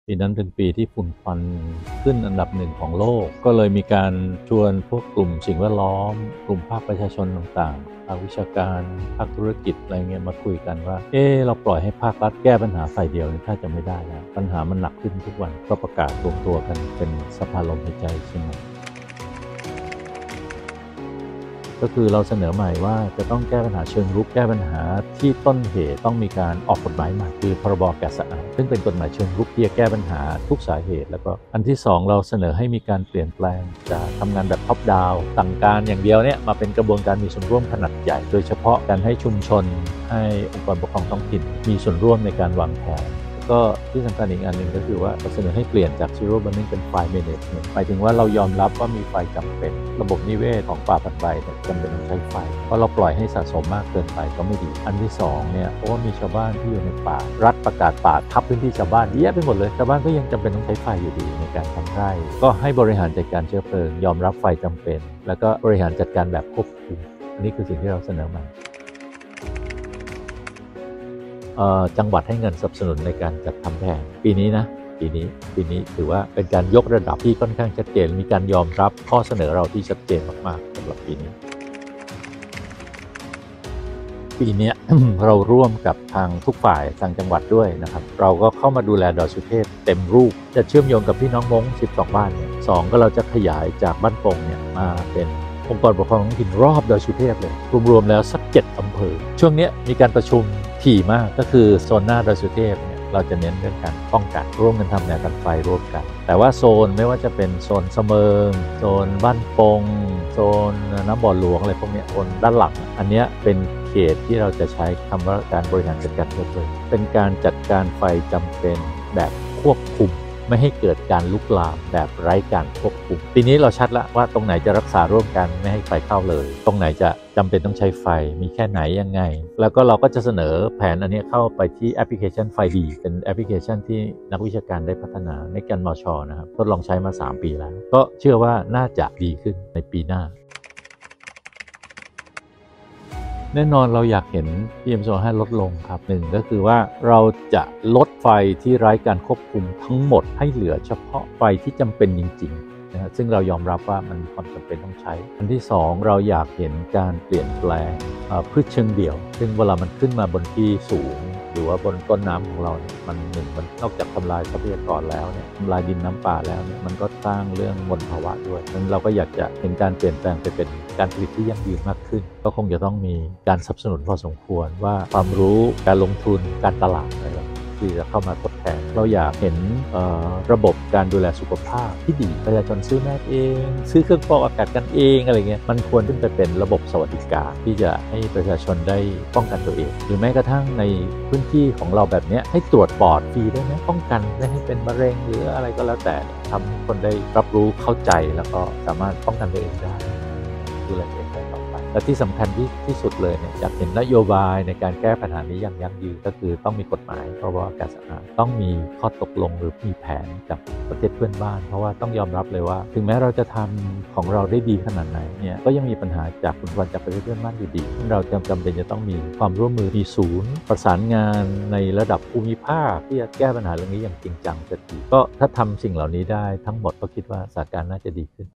ภาคเหนือรับมือ PM2.5 (3) เปลี่ยนจากงดเผาสิ้นเชิง เป็นคุมไฟ ใช้ไฟ
พูดคุยกับชัชวาล ทองดีเลิศ จากสภาลมหายใจเชียงใหม่ เมื่อภาคประชาสังคมในเชียงใหม่รวมตัวเรียกร้องสิทธิที่จะมีอากาศสะอาดหายใจ นำมาซึ่งข้อเสนอแก้ปัญหาเชิงรุก ทั้งข้อเสนอ พ.ร.บ. อากาศสะอาด ที่เพิ่งผ่านเข้าสภา การให้ชุมชนและองค์กรปกครองส่วนท้องถิ่นมีส่วนร่วมวางแผนก่อนถึงฤดูฝุ่นควัน และข้อเสนอให้เปลี่ยนจากงดการเผาสิ้นเชิง (Zero Burning) มาเป็นการควบคุมไฟ (Fire Management) โดยยอมรับว่าไฟนั้นจำเป็น ระบบนิเวศของป่าผลัดใบจำเป็นต้องใช้ไฟ ปล่อยให้สะสมมากไปก็ไม่ดี ในขณะที่ยอมรับการใช้ไฟในการทำเกษตร ที่ต้องบริหารจัดไฟแบบควบคุม

สถานการณ์ฝุ่นควัน PM2.5 หลายปีที่ผ่านมาอยู่ในขั้นเลวร้ายและกระจายไปทั่วประเทศ โดยเฉพาะภาคเหนือตอนบน ที่ฤดูแล้งทุกปีวัดค่าคุณภาพอากาศเข้าขั้นมลพิษสูงติดอันดับโลก ที่ผ่านมามีการเรียกร้องรัฐบาลให้มีมาตรการป้องกัน และดูแลประชาชนให้มากยิ่งขึ้น อย่างที่จังหวัดเชียงใหม่มีการเคลื่อนไหวเพื่อเรียกร้องสิทธิที่จะมีอากาศที่สะอาดหายใจติดต่อกันเป็นเวลาหลายปี อีกทั้งยังมีภาคประชาสังคมที่ออกมาทำบางอย่างเพื่อให้เกิดการเปลี่ยนแปลงกับเรื่องนี้อย่างจริงจัง

โดยรายงานข่าวภายใต้โครงการ ท้องถิ่นสร้างสื่อสอบ ชวนอ่านบทสนทนากับภาคประชาสังคม เอกชน และส่วนราชการในเชียงใหม่และจังหวัดภาคเหนือตอนบน เรื่องการรับมือปัญหามลพิษทางอากาศ รวมไปถึงมุมมองต่อการแก้ไขปัญหา ซึ่งจะแบ่งนำเสนอทั้งหมด 5 ตอน